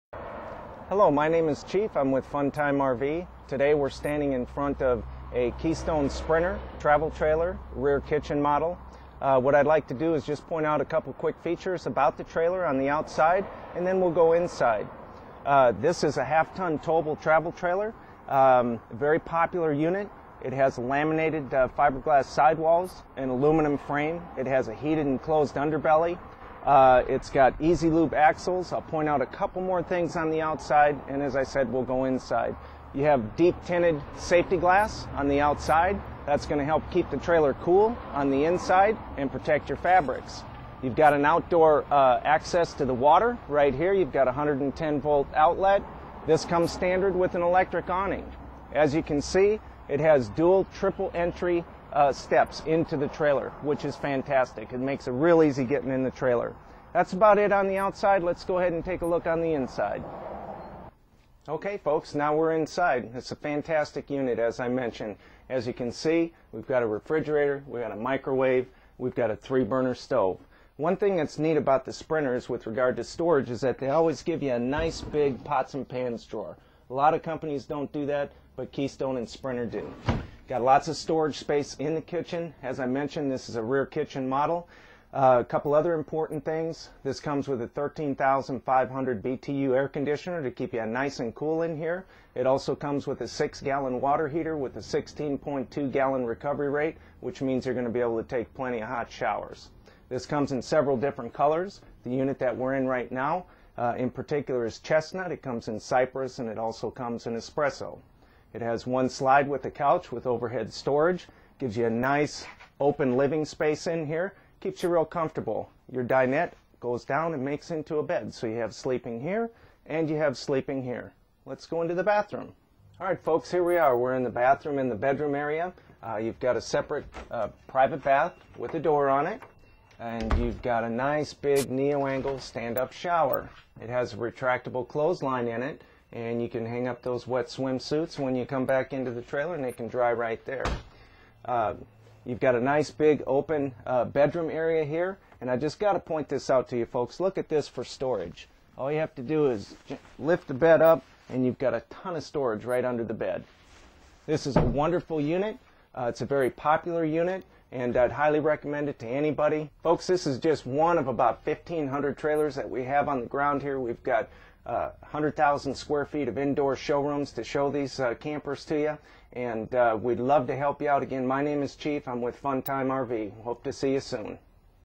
Sprinter 242RK Travel Trailer
This Sprinter RV is one of the most popular floorplans built. It has many nice features.